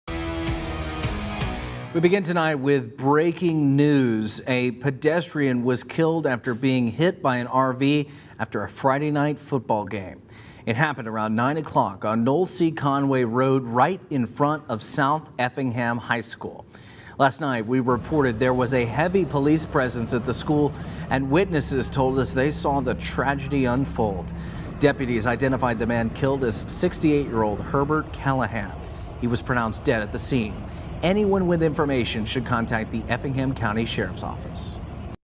68-year-old hit and killed by RV outside South Effingham High
68-year-old hit and killed by RV outside South Effingham High